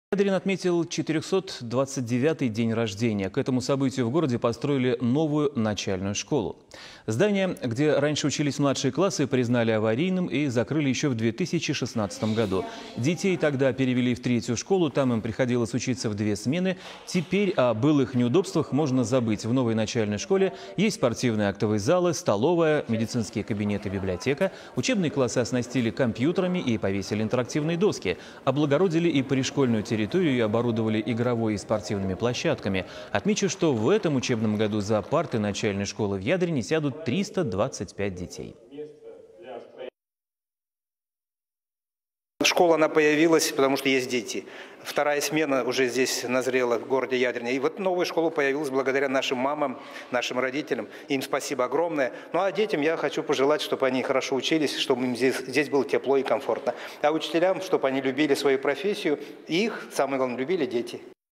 Подарок к учебному году: в Ядрине открыли новую начальную школу
Ядрин отметил 429-й день рождения. К этому событию в городе построили новую начальную школу.